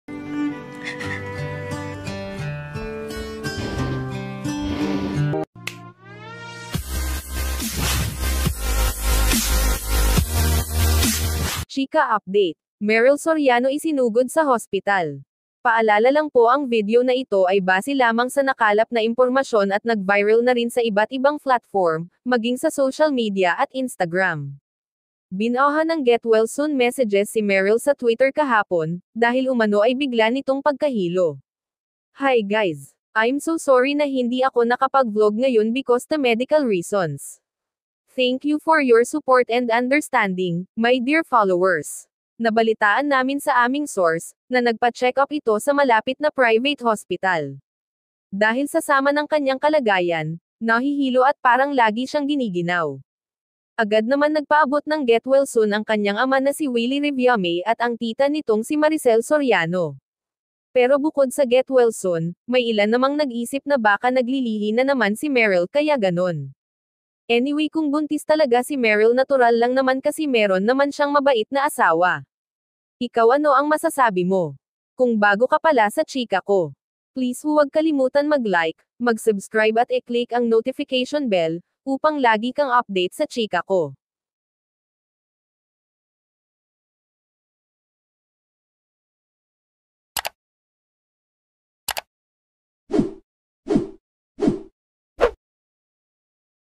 LAHAT NAIYAK:BUONG PAMILYA TODO IYAK MATAPOS ISINUGOD SA HOSPITAL ANG KANILANG MAHAL SA BUHAY!
bumuhos ang Luha maricel at Willie todo Iyak!

In this Video want to share the latest update IN SHOWBIZZ WORLD

Viral Today's!